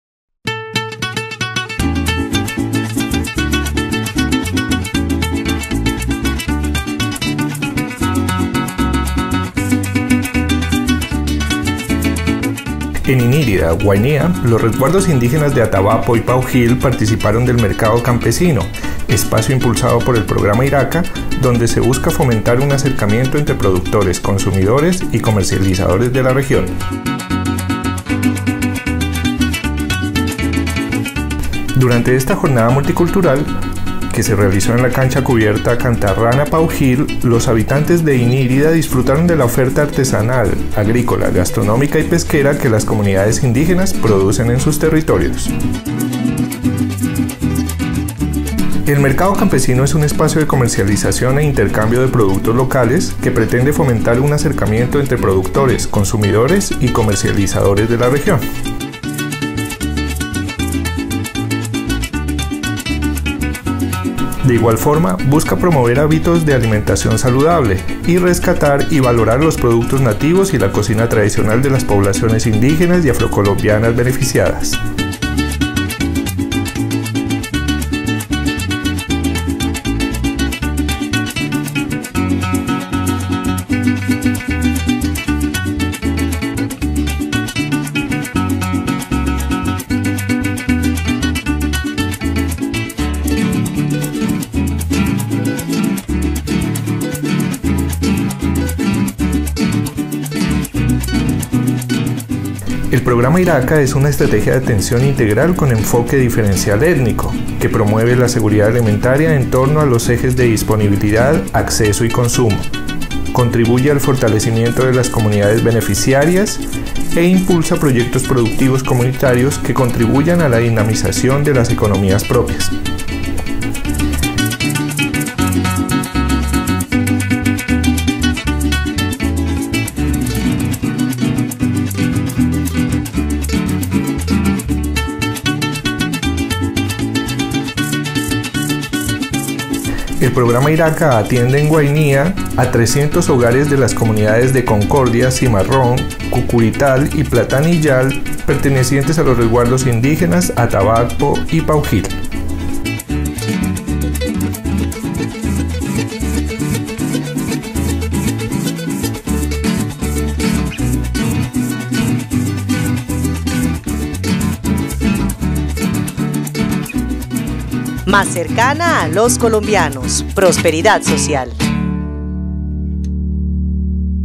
MERCADO CAMPESINO INIRIDA
En Inírida, Guainía, los resguardos indígenas de Atabapo y Paujíl participaron del Mercado Campesino, espacio impulsado por el programa IRACA, donde se busca fomentar un acercamiento entre productores, consumidores y comercializadores de la región.

Durante esta jornada multicultural – que se realizó en la cancha cubierta Cantarrana Paujil- los habitantes de Inírida disfrutaron de la oferta artesanal, agrícola, gastronómica y pesquera que las comunidades indígenas producen en sus territorios.

El Mercado Campesino es un espacio de comercialización e intercambio de productos locales, que pretende fomentar un acercamiento entre productores, consumidores y comercializadores de la región.

De igual forma, busca promover hábitos de alimentación saludables, y rescatar y valorar los productos nativos y la cocina tradicional de poblaciones indígenas y afrocolombianas beneficiarias.

El programa IRACA es una estrategia de atención integral con enfoque diferencial étnico, que promueve la seguridad alimentaria en torno a los ejes de disponibilidad, acceso y consumo; contribuye al fortalecimiento de las comunidades beneficiarias; e impulsa proyectos productivos comunitarios que contribuyan a la dinamización de las económicas propias.

El programa IRACA atiende a 300 hogares de las comunidades de Concordia, Cimarrón, Cucurital y Platanillal pertenecientes a los Resguardos indígenas Atabapo y Paujíl.